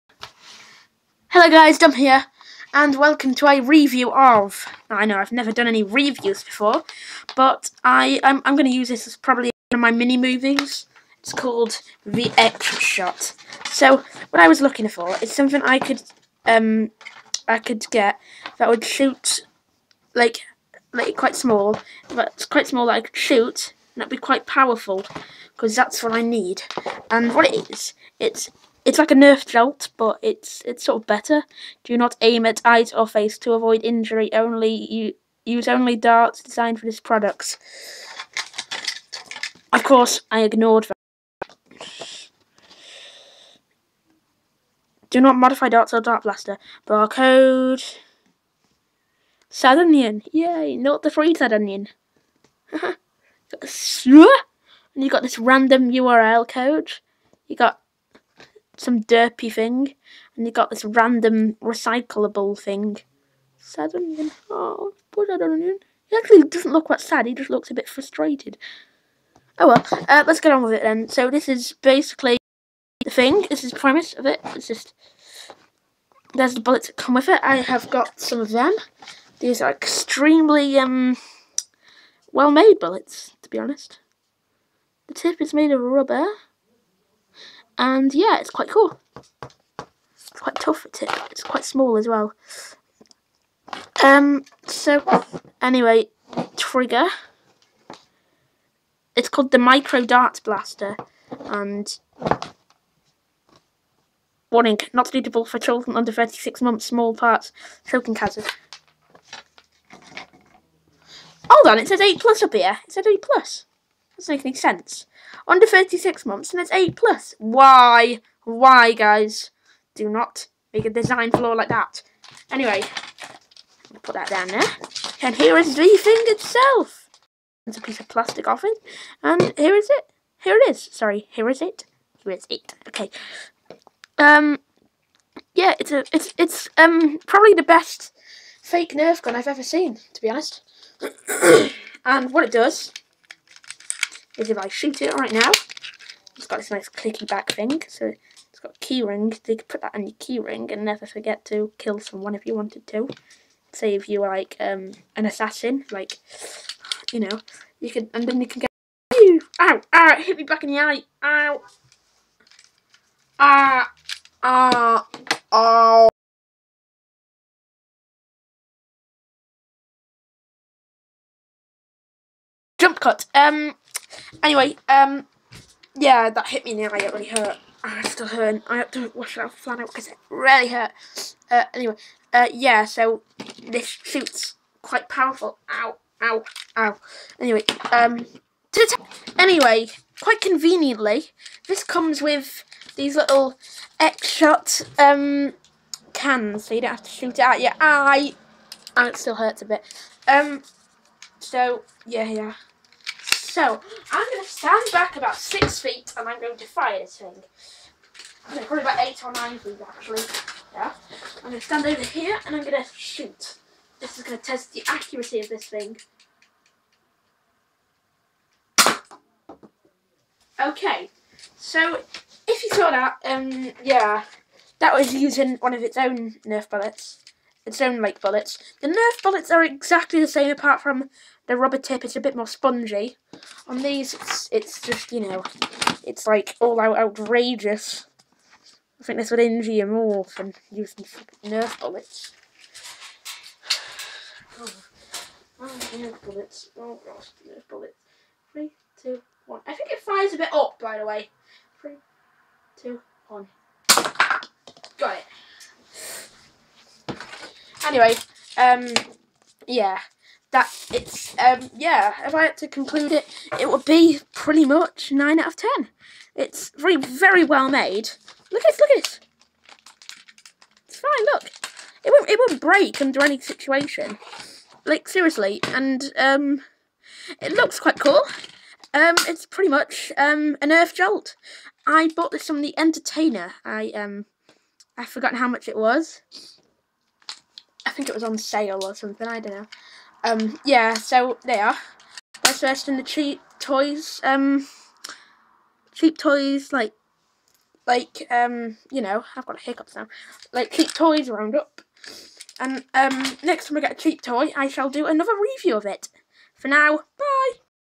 x shot micro review:better than Nerf jolt?
i think this is the best fake nerf i have come across in a long time,its absolutely amazing,it can fire normal nerf bullets,has excellent build quality,and costs like £3,someone send this to dr stuart ashen to review.